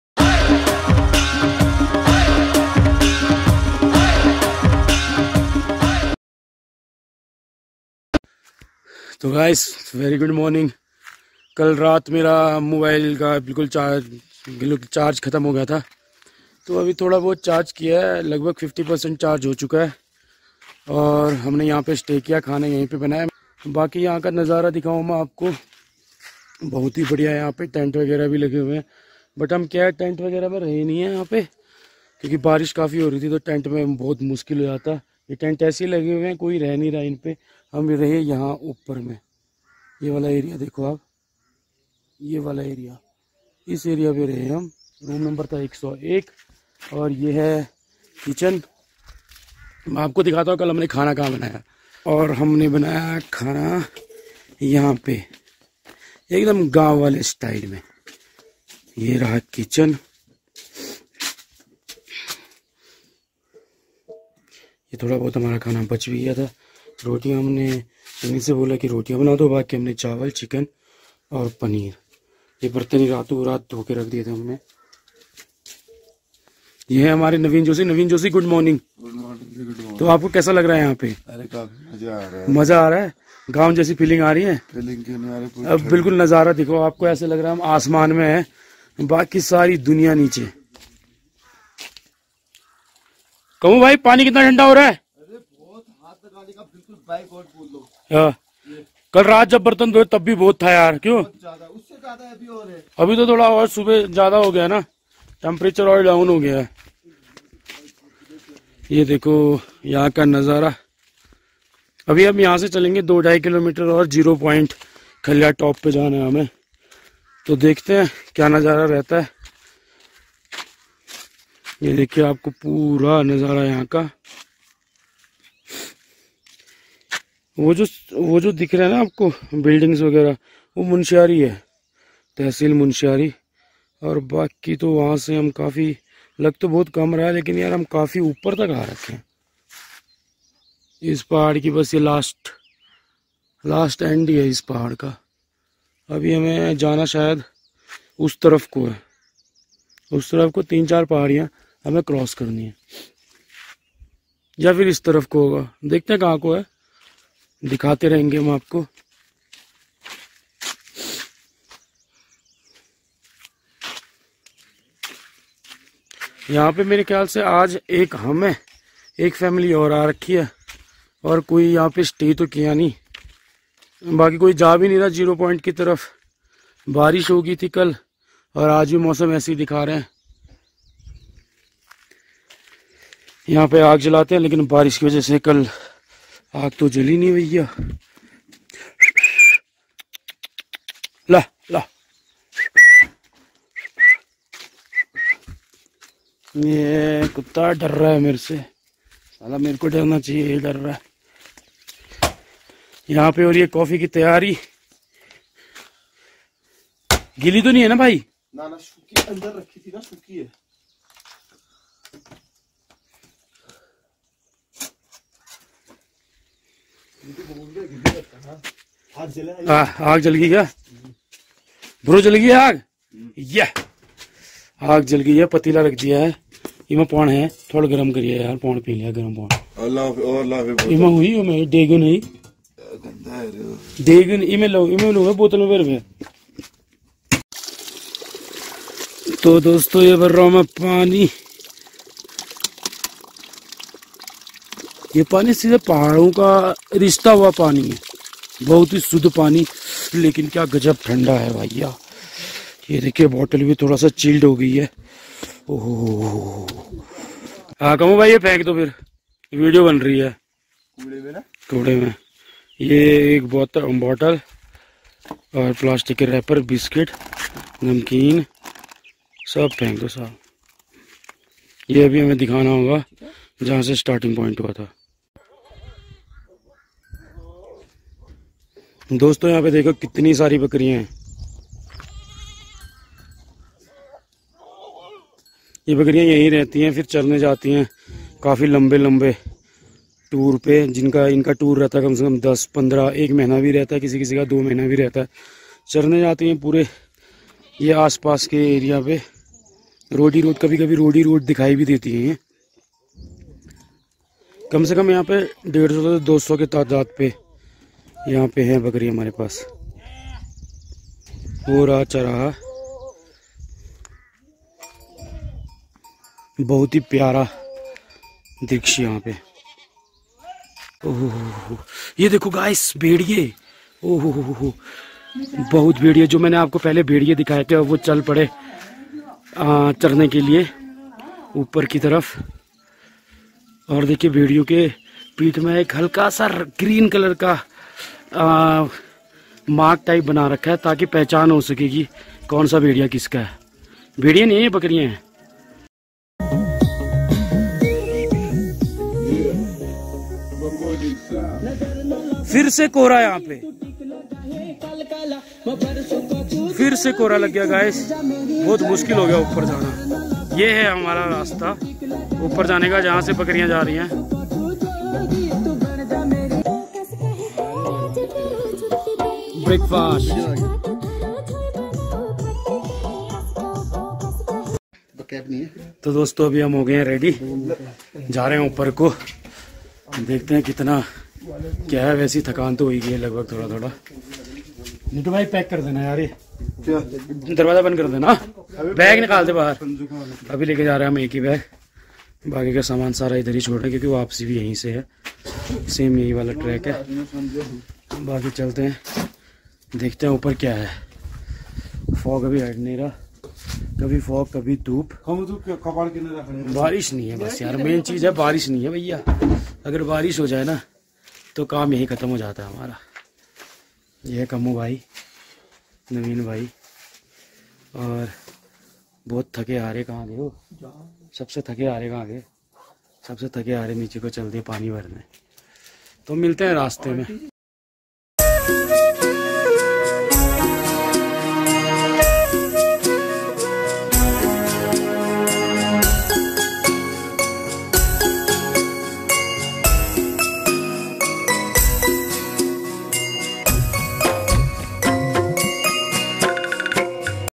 Khaliya Top: मुनस्यारी में है खलिया टॉप ट्रैक, समुद्र तल से ऊंचाई 3500 मीटर Munsiyari | Part 2
Khaliya Top Trek, Munsiyari
PART 2

Video recorded by - 1.